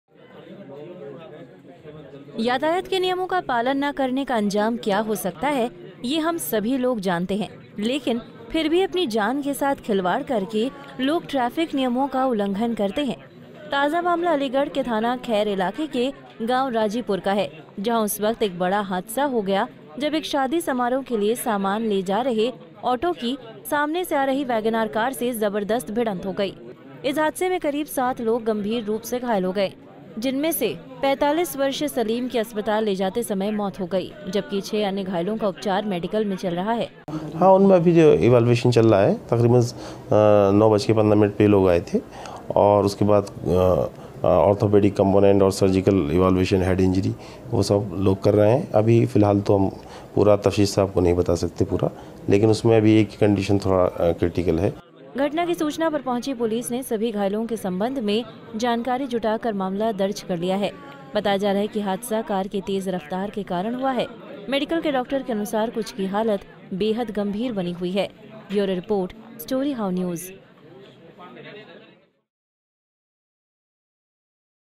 यातायात नियमों का पालन ना करने पर ऐसा हुआ अंजाम, गई मासूम की जान
यातायात के नियमों का पालन ना करने का अंजाम क्या हो सकता है ये हम सभी लोग जानते हैं, लेकिन फिर भी अपनी जान के साथ खिलवाड़ करके लोग ट्रैफिक नियमों का उल्लंघन करते हैं